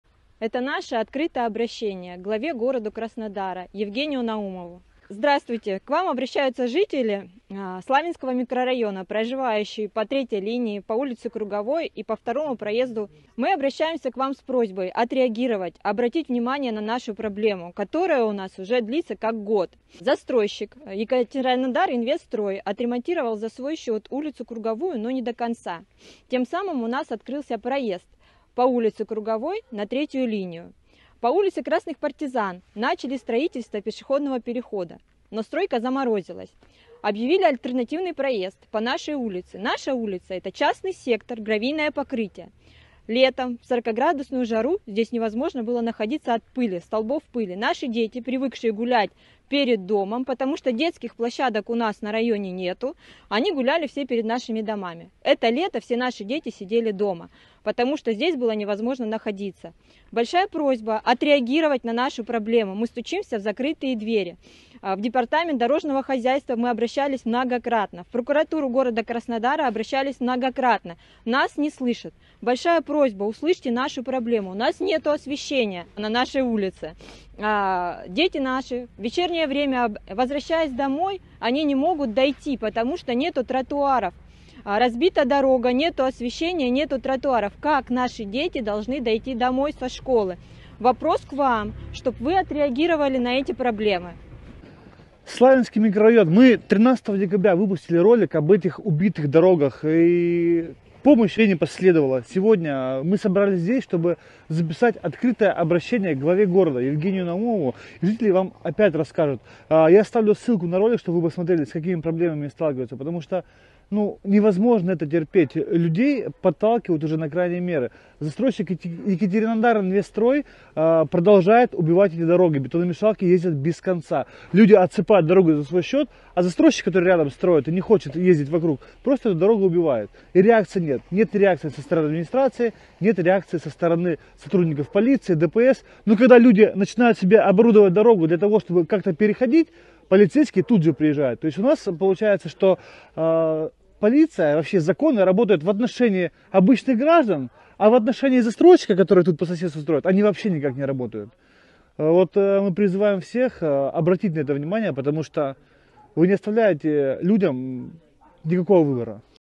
Убитые дороги Славянского микрорайона
Люди вышли на улицу с требованием благоустроить объездную дорогу улицы Красных Партизан в Краснодаре